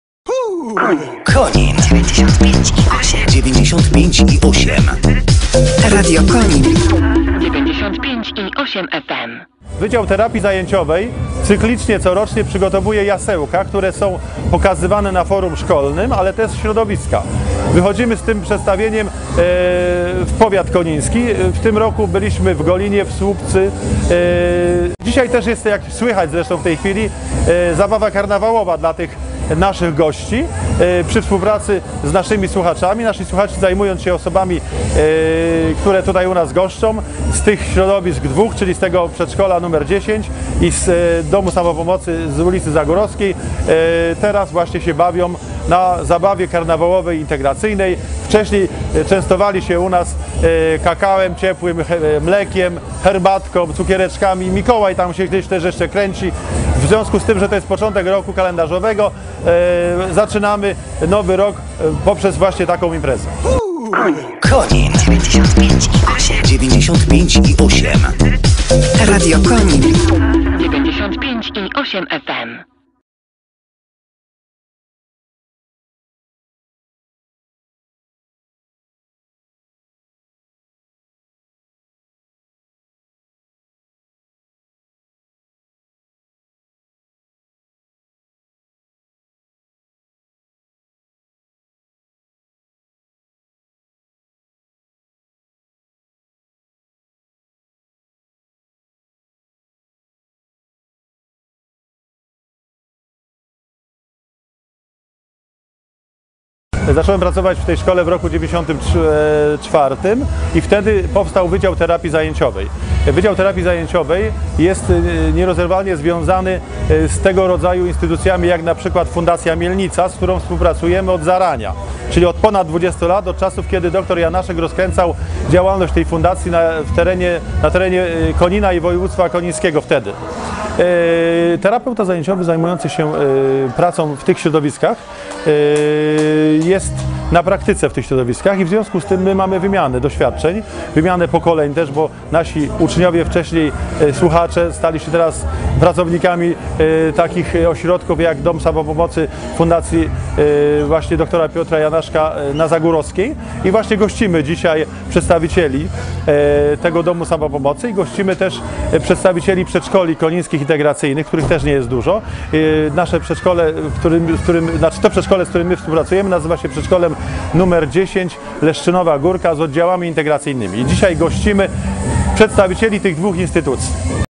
Jasełka i zabawa w Medyku
Od 1994 roku odbywaj się w Zespole Szkół Medycznych w Koninie imprezy integracyjne dla podopiecznych konińskich placówek terapeutycznych. O tegorocznej opowie dyrektor "Medyka" - Wojciech Szymczak.